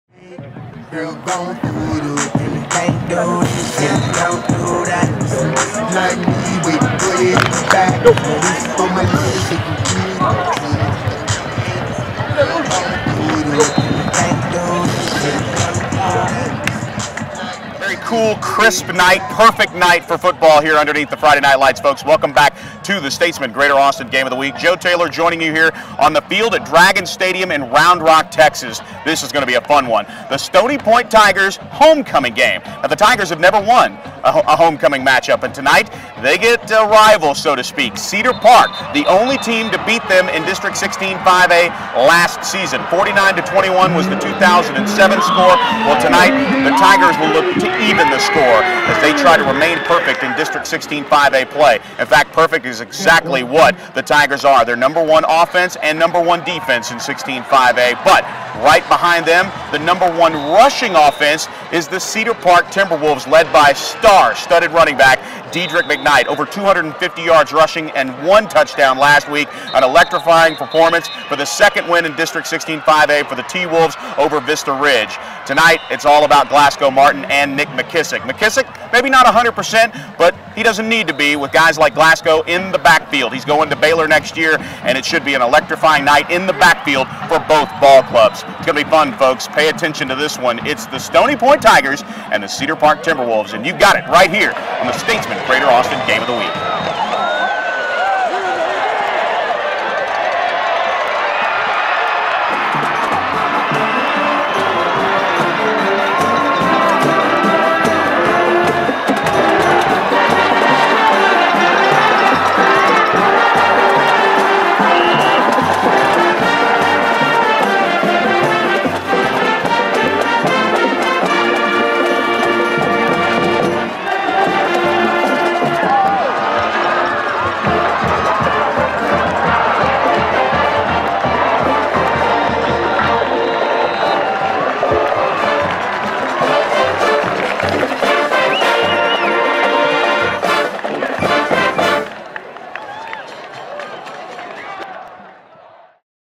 Game of the Week: Stony Point vs. Cedar Park
Stoney Point Tigers homecoming game against the Cedar Park Timberwolves at Dragon stadium in Round Rock.